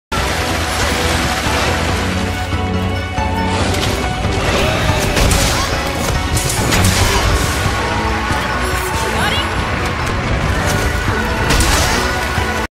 The first and only time I will ever use wall running effectively.
I carted shortly after, but don't tell anybody.